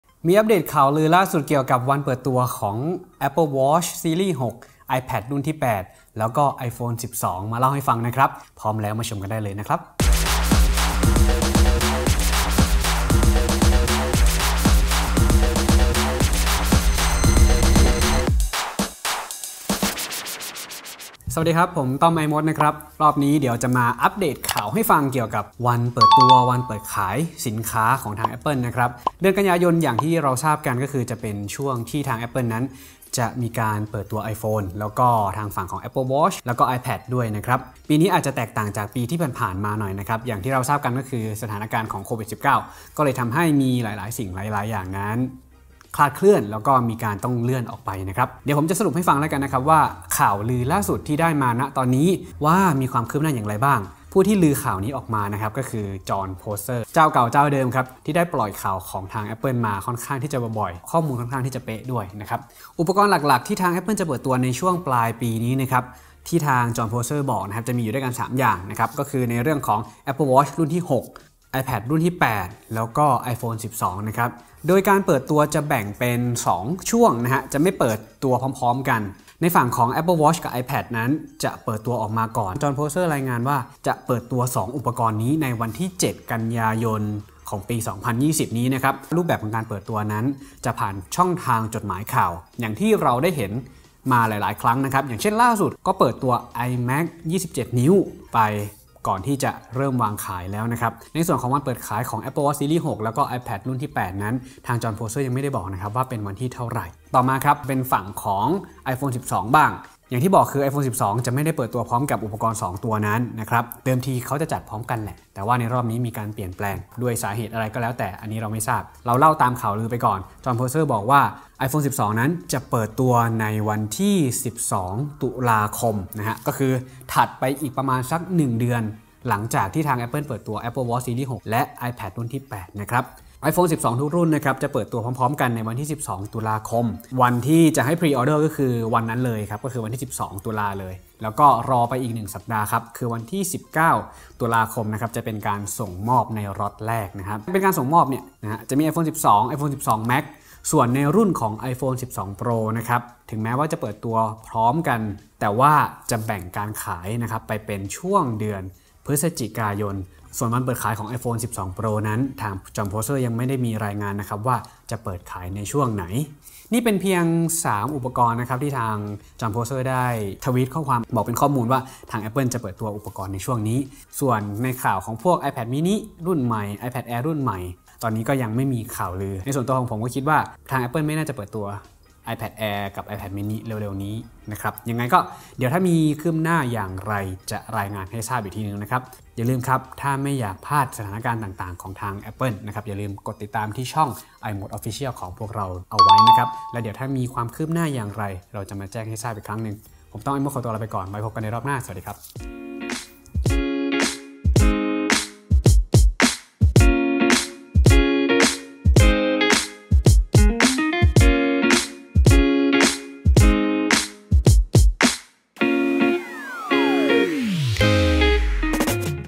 ข่าวลือ iPhone 12, iPad 8 และ Apple Watch Sereis 6 จะเปิดตัวไม่พร้อมกัน
Jon Prosser เผย Apple Watch, iPad เปิดตัว ก.ย. ส่วน iPhone 12 เปิดตัว ต.ค.
สำหรับข้อมูลกำหนดเปิดตัวสินค้าใหม่ที่รายงานโดย Jon Prosser มีดังนี้
- Apple Watch และ iPad เปิดตัวช่วงสัปดาห์ของวันที่ 7 ก.ย.
- iPhone 12 เปิดตัวช่วงสัปดาห์ของวันที่ 12 ต.ค.
- iPhone 12 เปิดให้สั่งซื้อช่วงสัปดาห์ของวันที่ 12 ต.ค., เปิดขายช่วงสัปดาห์ของวันที่ 19 ต.ค.
- iPhone 12 Pro ยังไม่มีกำหนดว่าวันไหน แต่น่าจะอยู่ในช่วงเดือน พ.ย.

หากมีอัปเดตจะแจ้งให้ทราบอย่างต่อเนื่อง
🔴 อย่าลืมชมข้อมูล สรุปข่าวลือ iPhone 12 เดือน ส.ค. 2020 เปิดตัวช้ากว่าเดิม, รองรับ 5G, มีให้เลือก 4 รุ่น